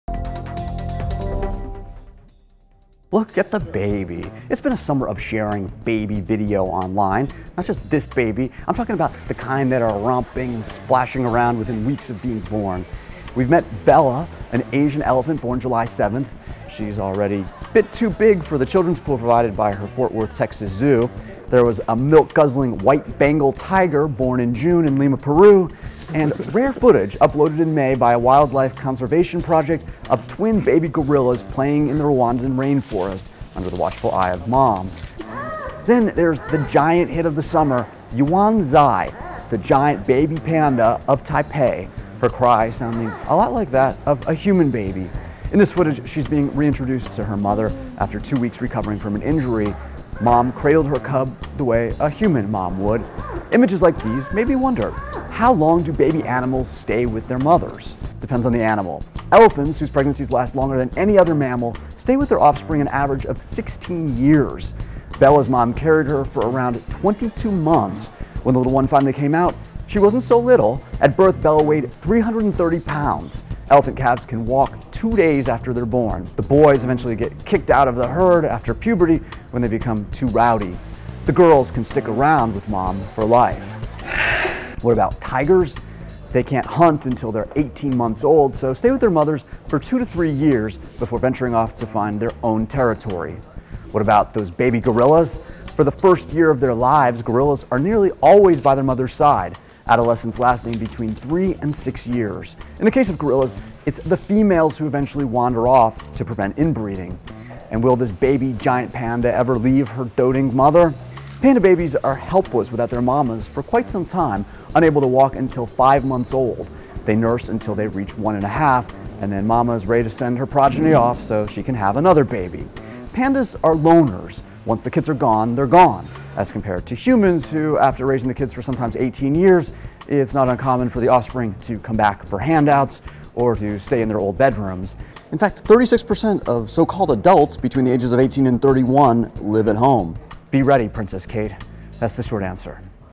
How Long Do Baby Mammals Stay With Their Mothers? | Baby Animals
Videos of newborn animals are going viral this summer, prompting WSJ's Jason Bellini to wonder how long these animals stay with their mothers. Here's TheShortAnswer.